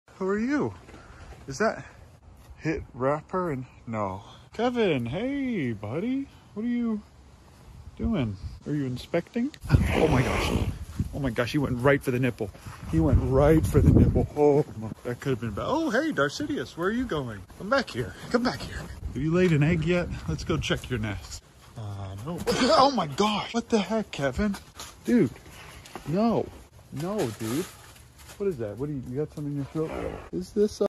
Kevin Performs A Surprise Attack (Jump Scare Warning ⚠️😳)
Uncle Ben is disappointed to find its Kevin not Dababy. while looking to see if karen laid an egg Kevin jumps out with an attack